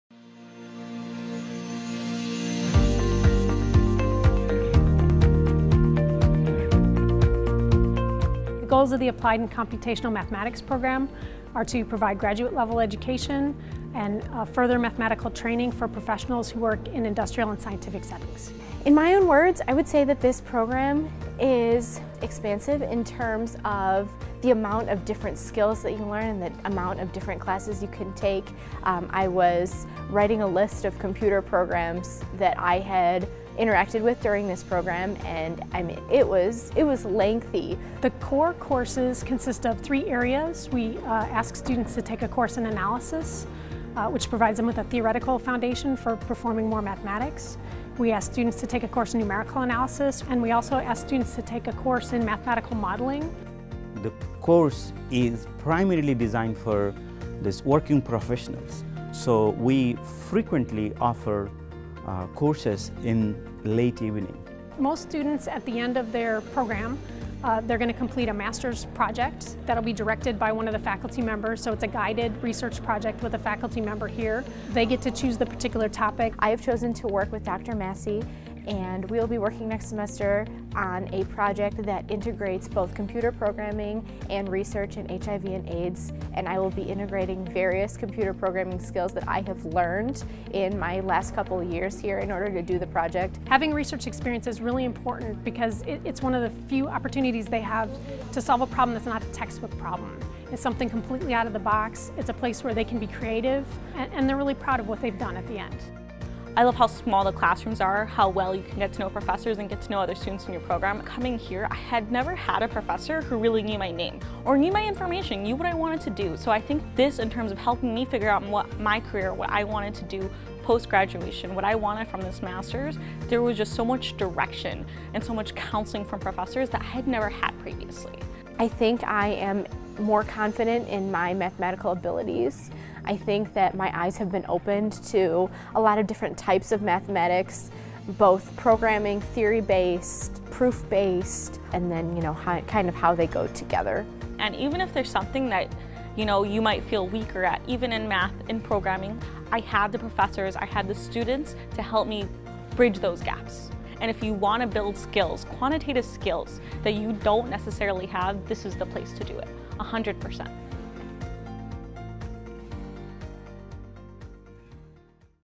UM-Dearborn MS in Applied & Computational Math
The MS in Applied & Computational Math provides graduate-level education in applied mathematics in order to develop comprehension of principles of applied mathematics and skills in employing those principles in industrial or scientific settings.